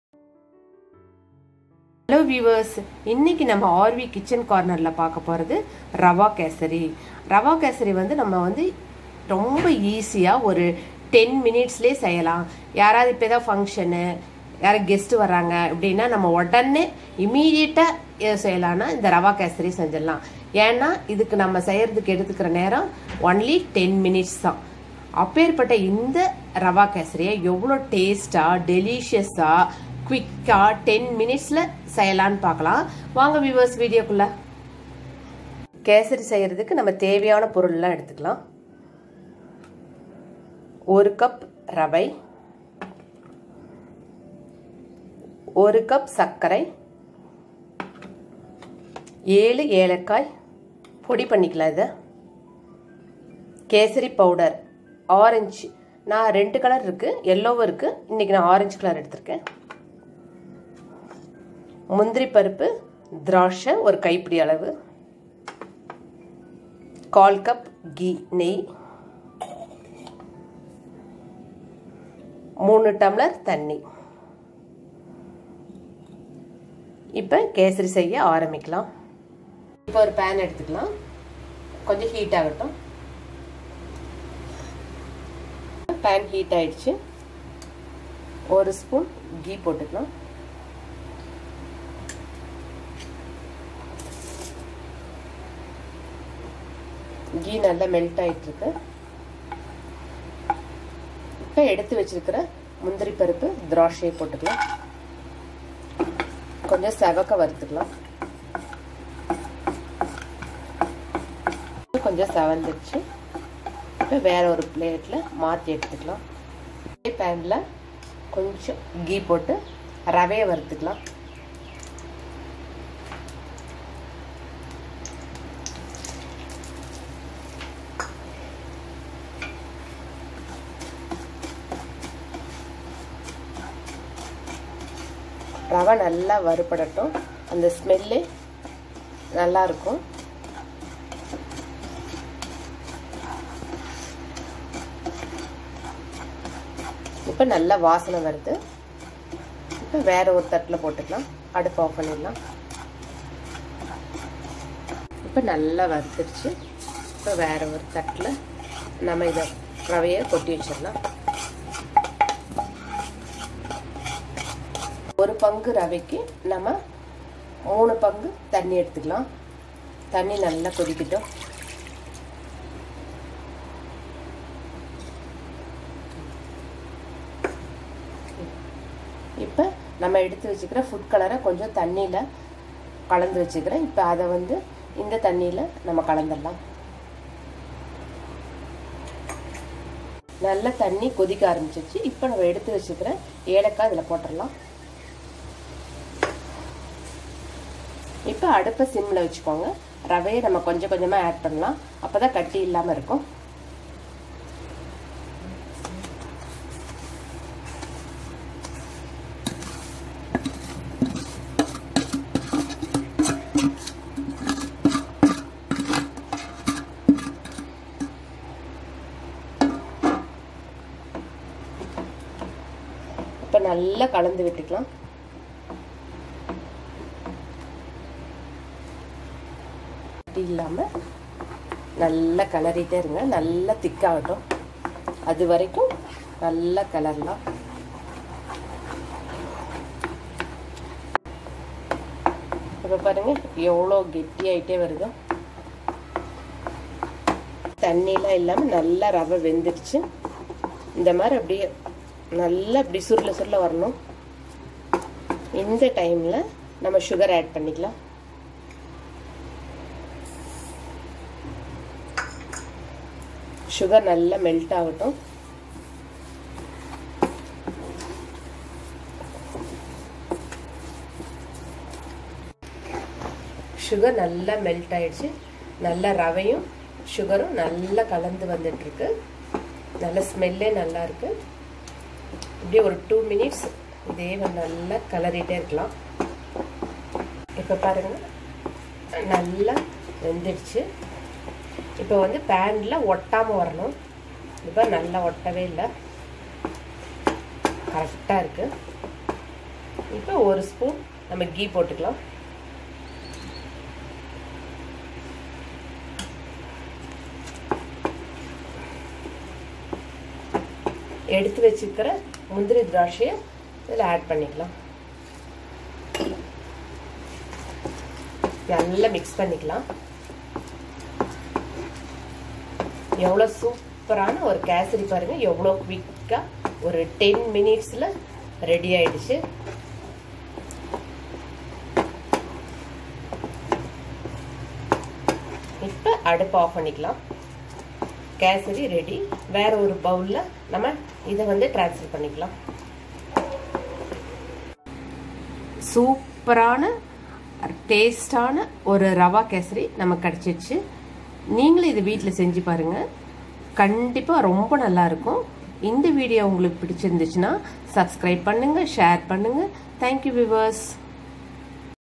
How to make Rava Kesari in Tamil | ரவா கேசரி | kalyana Kesari recipe in Tamil
Tasty rava kesari / kalyana kesari in 15 mins - traditional sweet